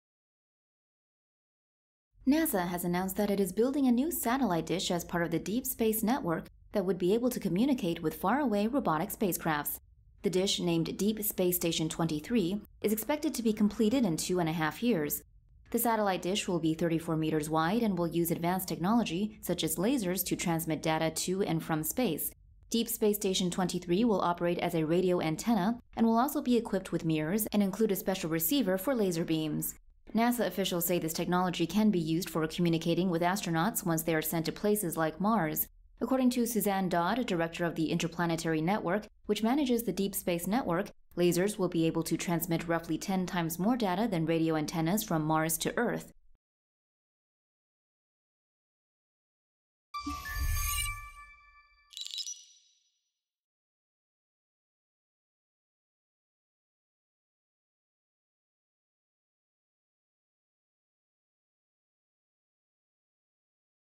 NASA to use lasers space communication
NASA has announced that it is building a new satellite dish as part of the Deep Space Network that would be able to communicate with far away robotic spacecrafts.

The dish, named Deep Space Station-23, is expected to be completed in two and a half years.

The satellite dish will be 34 meters wide and will use advanced technology, such as lasers, to transmit data to and from space. Deep Space Station-23 will operate as a radio antenna and will also be equipped with mirrors and include a special receiver for laser beams.

NASA officials say this technology can be used for communicating with astronauts once they are sent to places like Mars.

According to Suzanne Dodd, director of the Interplanetary Network, which manages the Deep Space Network, lasers will be able to transmit roughly 10 times more data than radio antennas from Mars to Earth.

NASA currently has deep space networks in Goldstone, California; Madrid, Spain; and Canberra, Australia. This is to allow spacecraft to have continual contact with Earth while our planet rotates.

As part of the Artemis program, NASA plans to launch three lunar missions by 2024 which will eventually pave way for astronauts to make their way to Mars and make human exploration of the red planet a reality.

RUNDOWN SHOWS:
1. New satellite dish
2. Dimensions of the new satellite dish
3. How it would work

"The satellite dish will be 34 meters wide and will use advanced technology, such as lasers, to transmit data to and from space."

"Deep Space Station-23 will operate as a radio antenna and will also be equipped with mirrors and include a special receiver for laser beams."